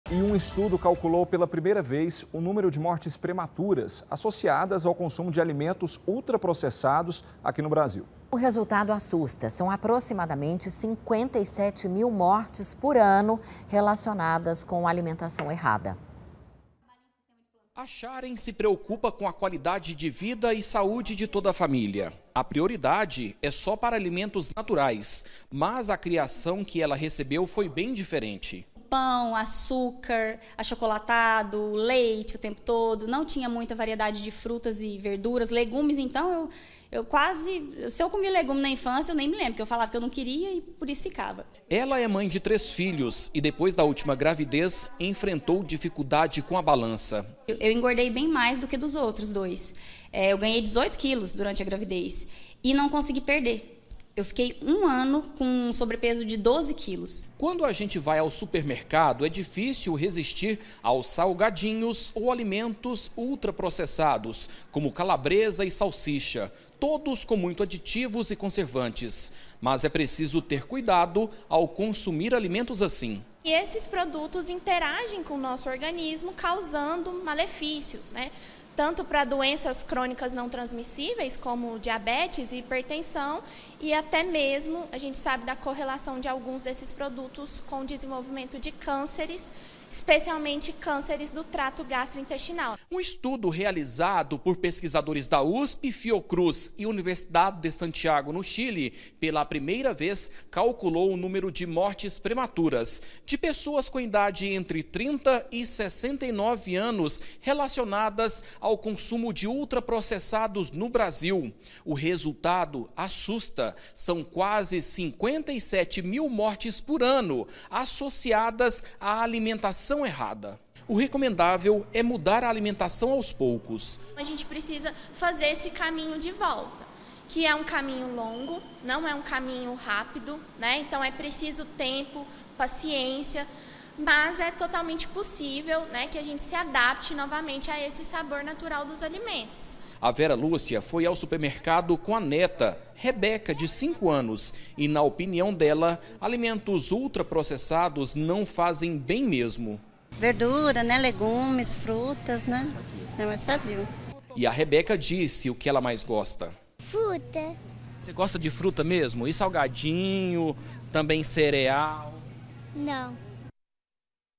ALIMENTOS ULTRAPROCESSADOS: ALIMENTAÇÃO ERRADA ESTÁ LIGADA A CASOS DE MORTES
UM ESTUDO CALCULOU PELA PRIMEIRA VEZ O NÚMERO DE MORTES PREMATURAS ASSOCIADAS AO CONSUMO DE ALIMENTOS ULTRAPROCESSADOS NO BRASIL. O RESULTADO ASSUSTA: SÃO APROXIMADAMENTE 57 MIL MORTES POR ANO, RELACIONADAS COM ALIMENTAÇÃO ERRADA.